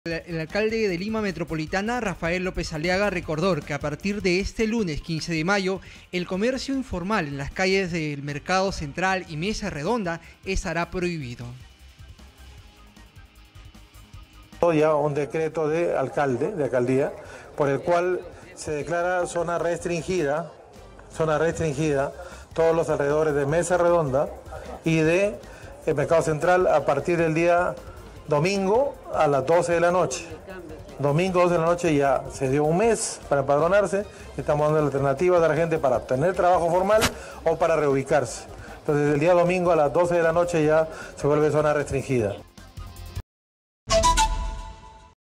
🔴🔵Rafael López Aliaga asegura que no habrá ambulantes en Mesa Redonda y Mercado Central
El alcalde de Lima, Rafael López Aliaga, asegura que desde mañana 15 de mayo, Mesa Redonda y Mercado Central estarán sin ambulantes.

Exitosa Perú
"Exitosa: La voz que integra al Perú"

Corporación Universal